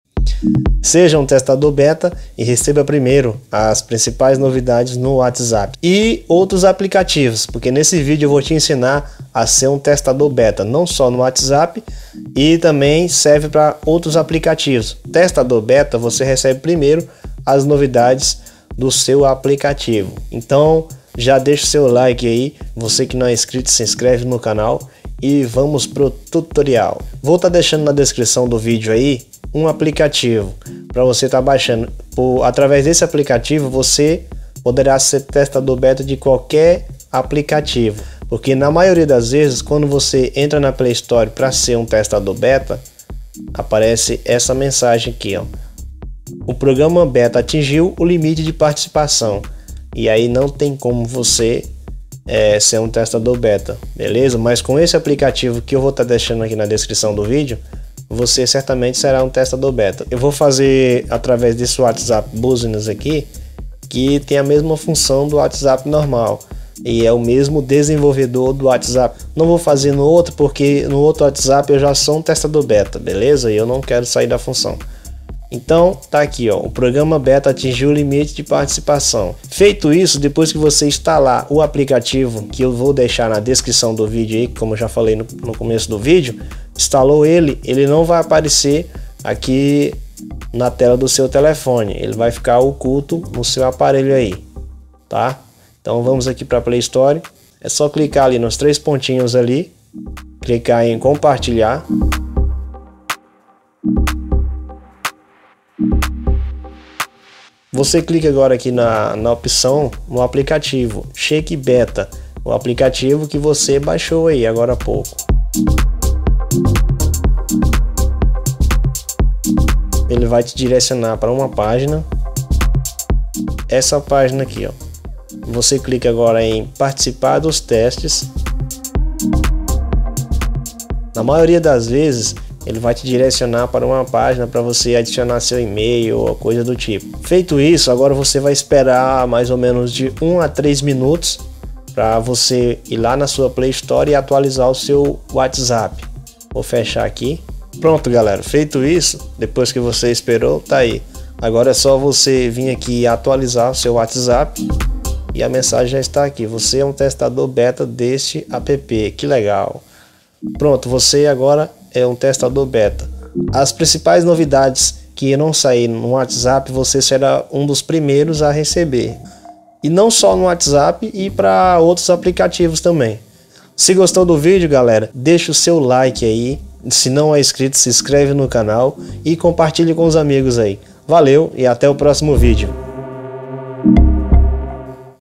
Receba Novidades do Whatsapp Primeiro VEJA COMO SER TESTADOR BETA
Neste Vídeo ensino como ser testador beta do whatsapp e outros aplicativos android.